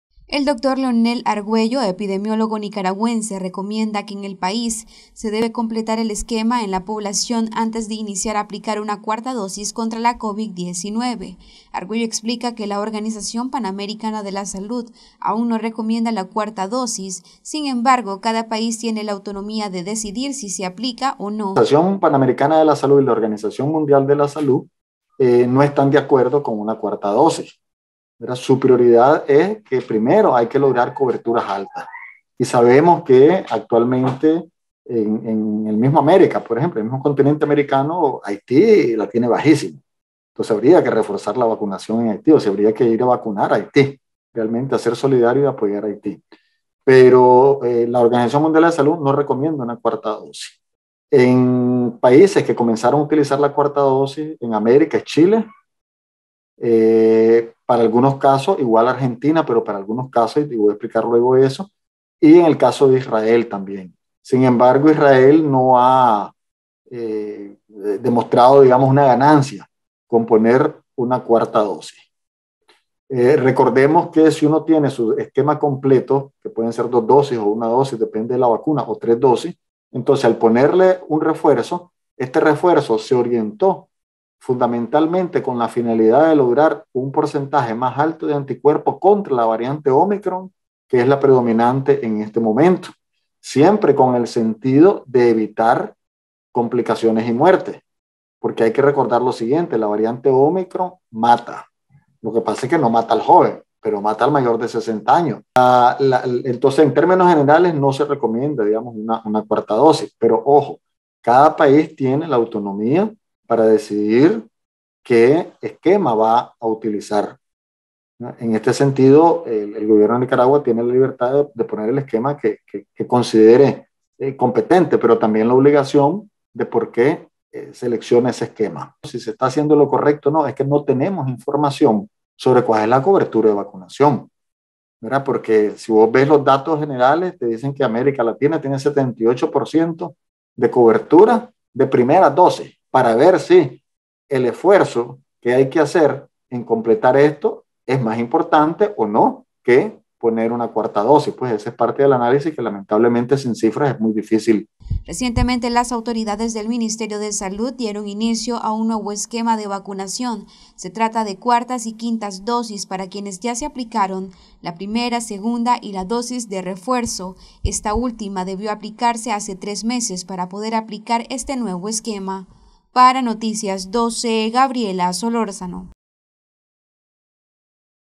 Epidemiólogo recomienda completar esquemas antes de iniciar a aplicar una 4ta dosis anticovid
Noticias12Nicaragua Epidemiólogo recomienda completar esquemas en población antes de iniciar a aplicar una cuarta dosis contra la covid-19, La OPS aún no recomienda la 4ta dosis sin embargo cada país tiene autonomía de decidir si se aplica o no